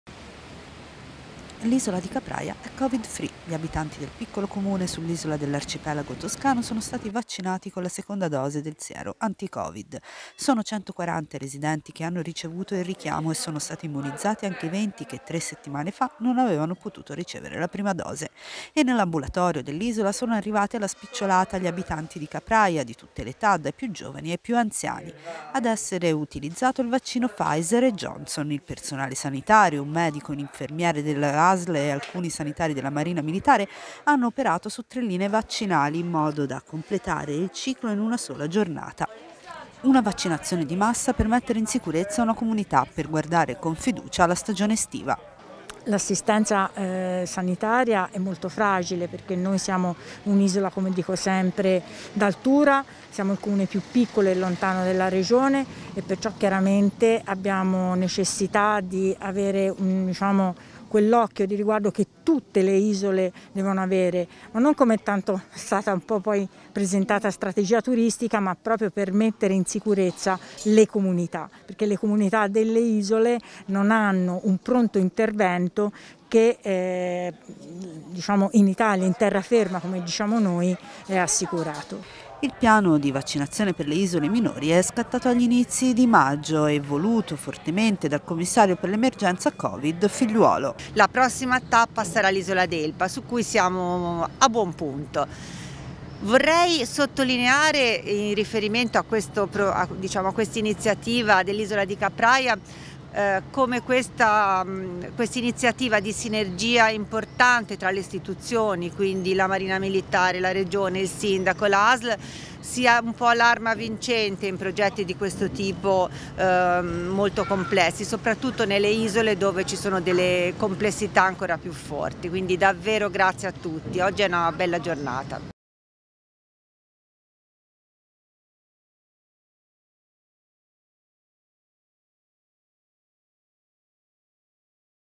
L'Isola di Capraia è Covid free
Completato dal personale medico e infermieristico delle Asl Nord Ovest in collaborazione con gli operatori sanitari della Marina Militare il ciclo vaccinale di tutti i 105 residenti nell'isola con la somministrazione dei sieri Pfizer, AstraZeneca e Janssen. Capraia è fra le prime isole italiane Covid free e questo consente di mettere in sicurezza tutti i cittadini che vivono e lavorano nell'isola ed è il primo passo per la realizzazione del piano iniziato nei primi giorni di maggio che prevede l'immunizzazione di tutti gli abitanti delle isole minori e che si concluderà nel prossimo luglio con il completamento delle vaccinazioni anche a Giannutri e al Giglio, di competenza territoriale della Asl sud est. La notizia